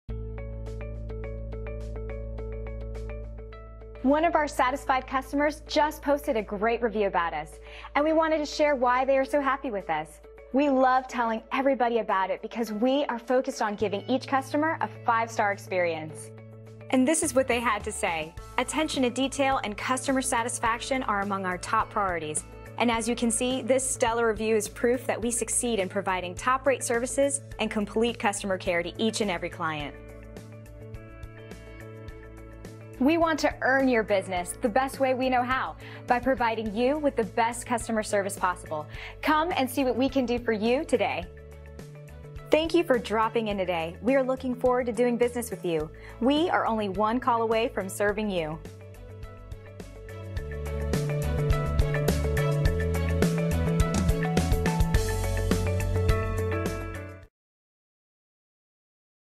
Bonness Cosmetic Surgery & Spa Vita Brookfield WI Excellent 5 Star Review
Reviews
        Excellent 5 Star Review

        Dr. Bonness made me feel very comfortable.  I had consulted with many doctors.  When I met her I knew she was the One.  Dr. Bonness and staff have always been friendly and kind.  Thank you Dr. Bonness.  You have given my confidence back.  I cannot thank you enough.

        Bonness Cosmetic Surgery & Spa Vita
        20320 Greenfield Ave.
        Brookfield WI
        53045